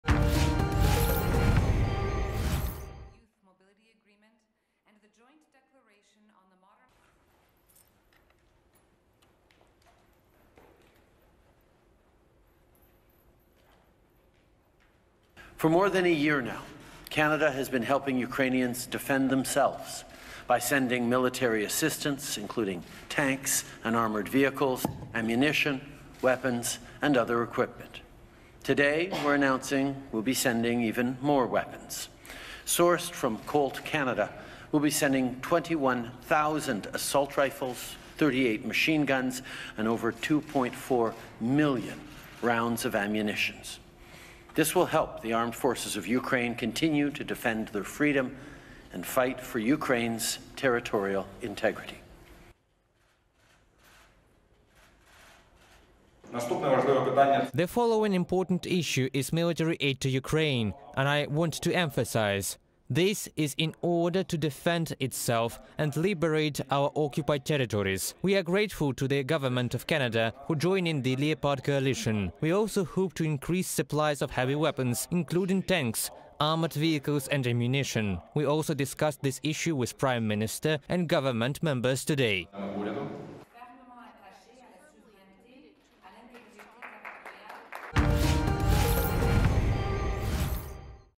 21 000 Colt assault rifles: more military aid from Canada to Ukraine – PMs meeting
Canada is expanding support for Ukraine: economic and military. Prime Minister Denys Shmyhal and Prime Minister Justin Trudeau signed the renewed free trade agreement in Canada. See how it was – in our video.